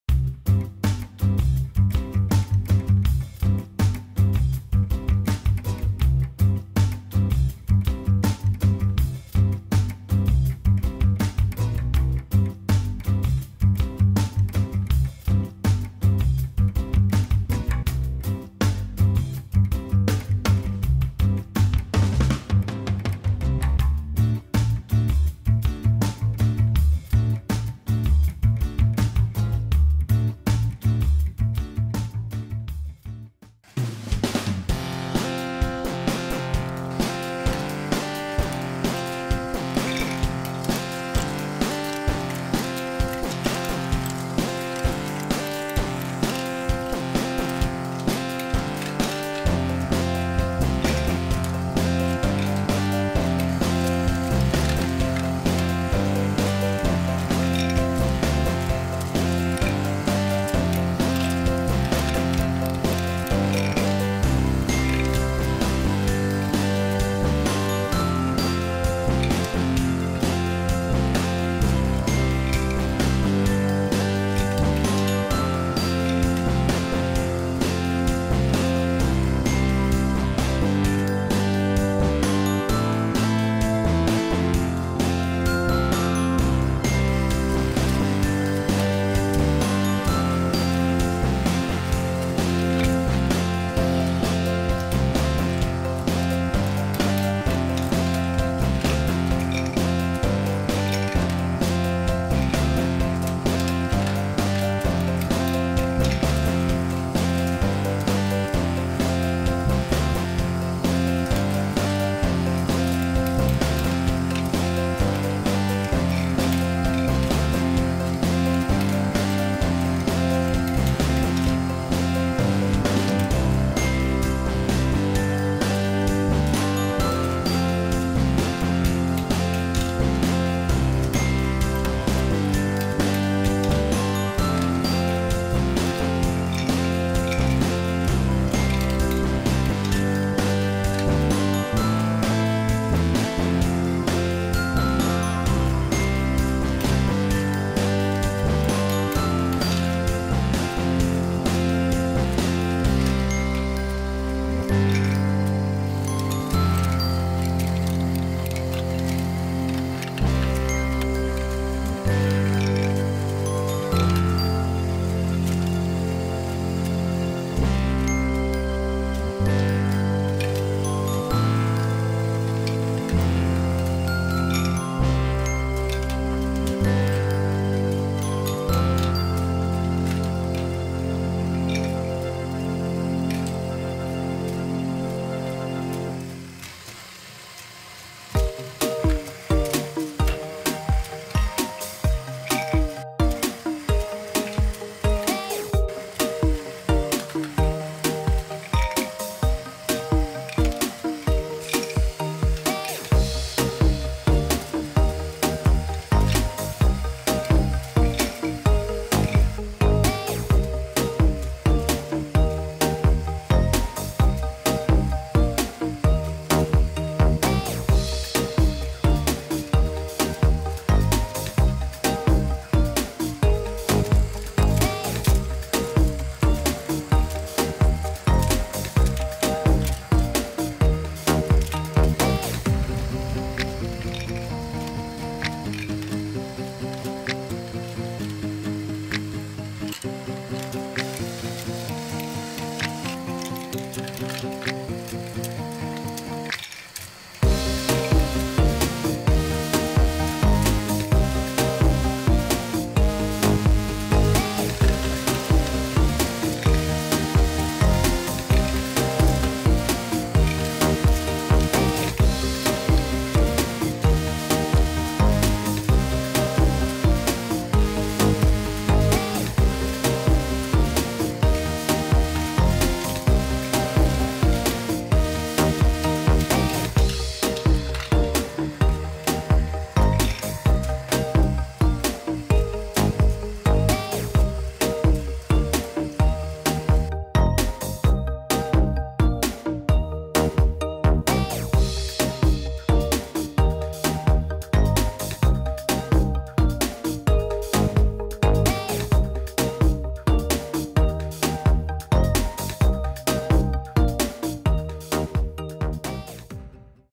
My First 'Great' Ball Contraption
This week I made a GBC device thingy, my first attempt at a great ball contraption was a real adventure of try, fail, try again, fail again but in the end, I am really happy with the result and I hope you will enjoy it as much as I did building it.